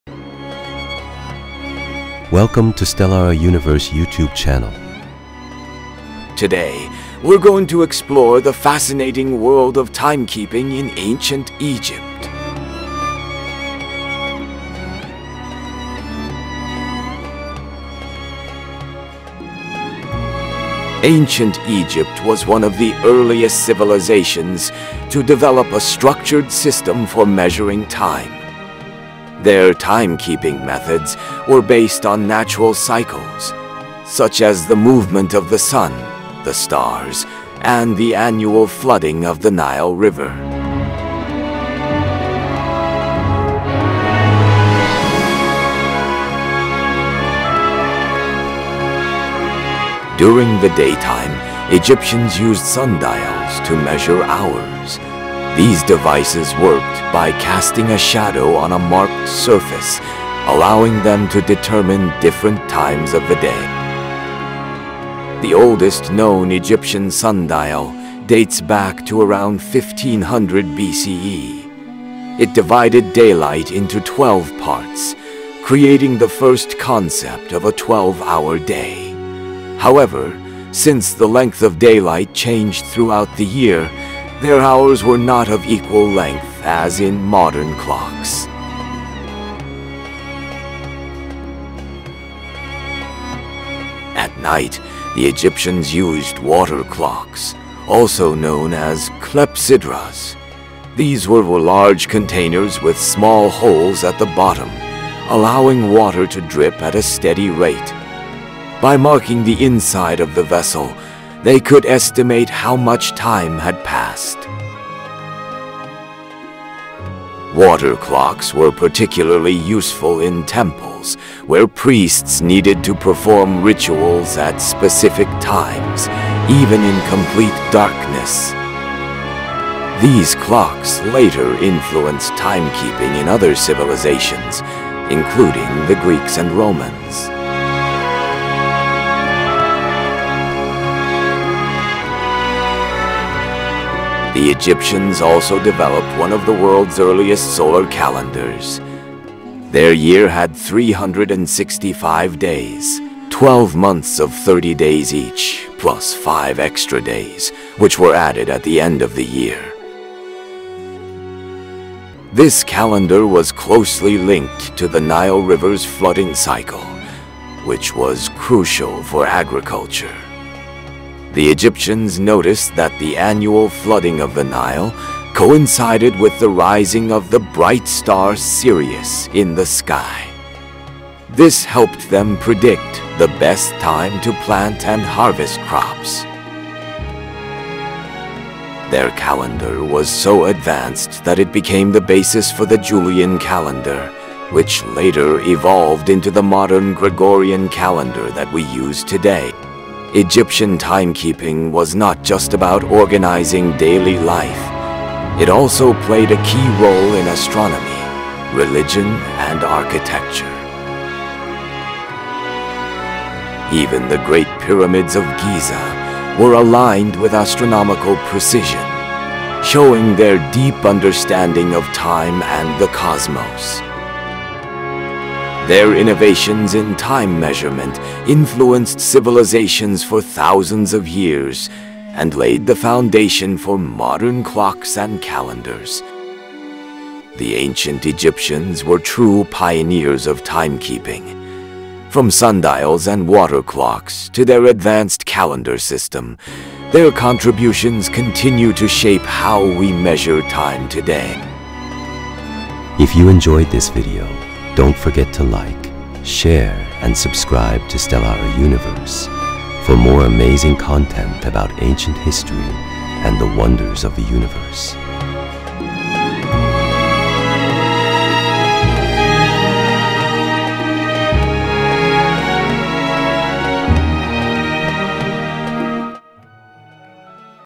Timekeeping in Ancient Egypt
### **📜 Timekeeping in Ancient Egypt – How They Measured Time**

Welcome to **Stellara Universe!** 🌌 Today, we journey back in time to explore how the **Ancient Egyptians** measured time using **sundials and water clocks**. ⏳🏺

The **Egyptians were pioneers in timekeeping**, developing some of the earliest methods to track the hours of the day and night. During the **day**, they used **sundials**, relying on the movement of the sun’s shadow to divide time. At **night**, they used **water clocks (clepsydras)**, allowing priests and scholars to measure time with the steady flow of water.

🌞 How Egyptians used **sundials** to track time during the day
🌊 The role of **water clocks** in night-time timekeeping
📅 The **Egyptian calendar** and its influence on modern calendars
🌠 How astronomy played a key role in their time measurement

From **religious ceremonies** to **agriculture** and **pyramid construction**, timekeeping was an essential part of Egyptian civilization. Their innovations helped shape the way we measure time even today!

📽️ **Watch now and uncover the secrets of ancient timekeeping!**